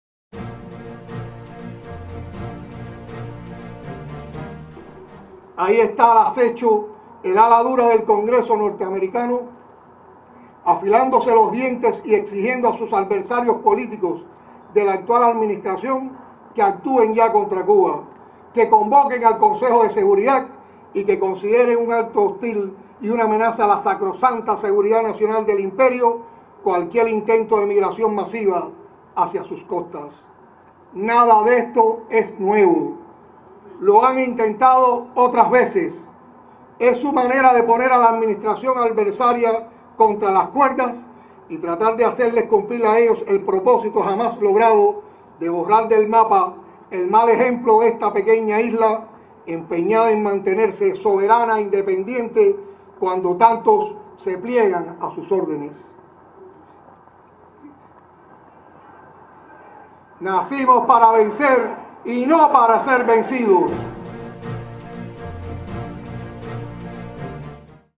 Por la libertad conquista, por la Patria
Ingenuos aquellos que nos ofrecen fórmulas entreguistas Cuba es de los cubanos y para resolver nuestras dificultades trabajamos cada día Levanten el bloqueo, es una petición mundial